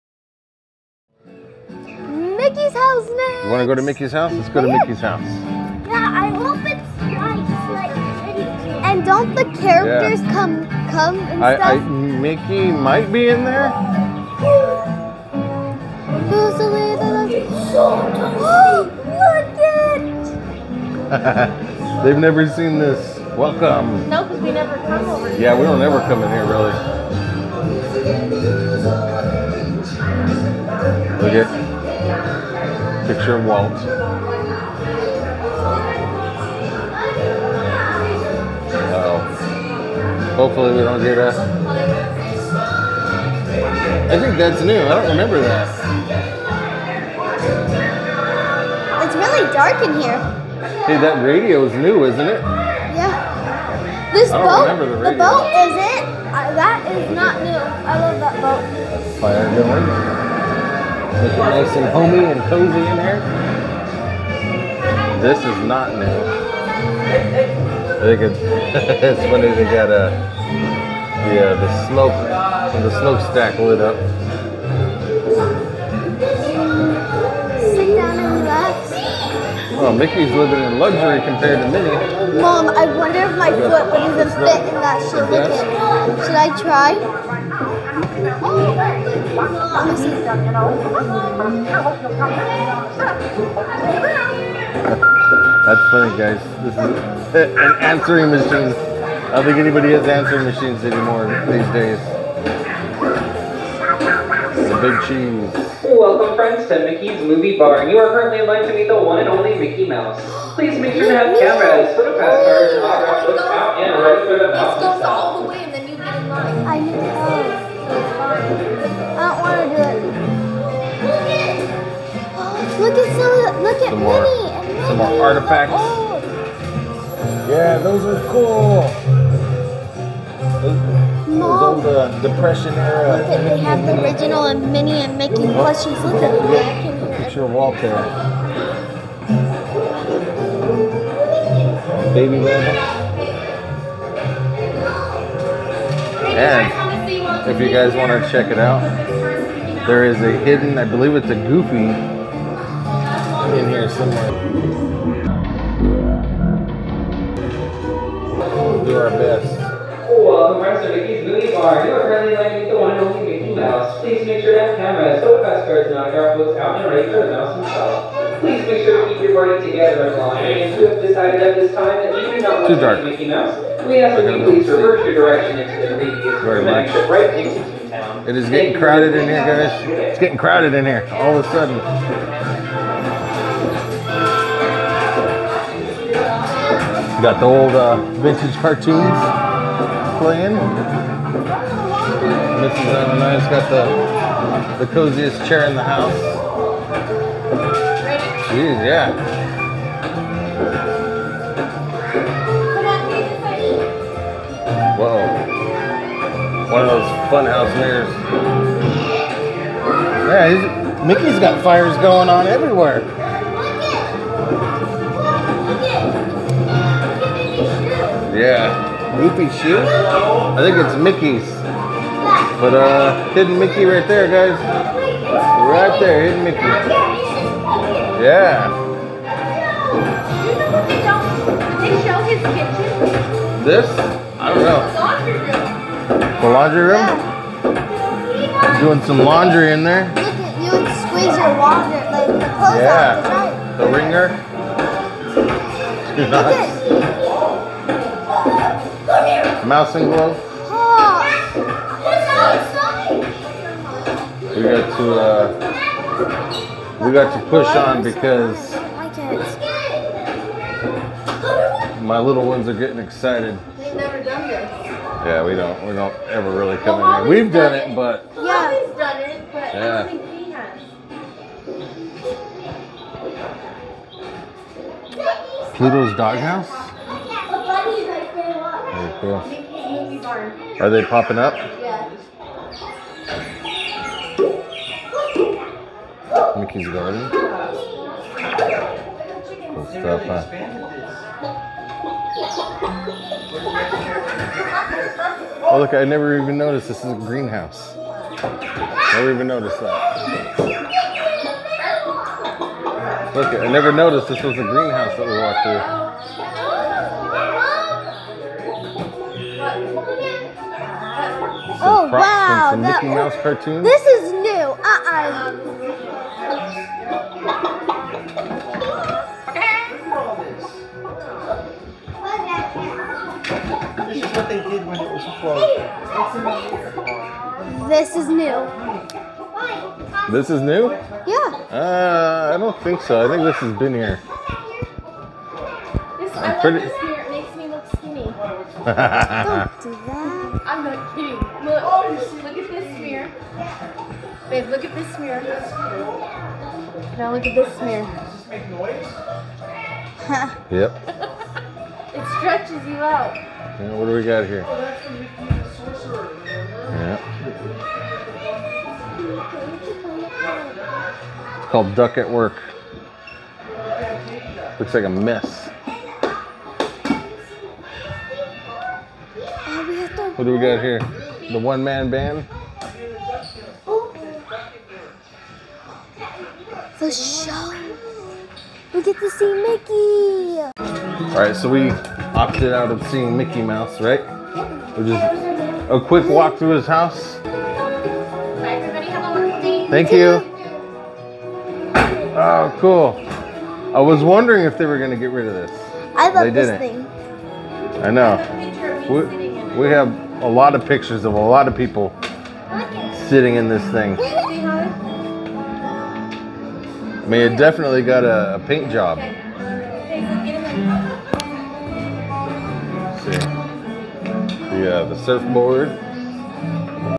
Mickey's House Walk-through | Mickey's Toontown Disneyland
Join us for a walk-through of Mickey Houst in Toontown at The Disneyland Resort.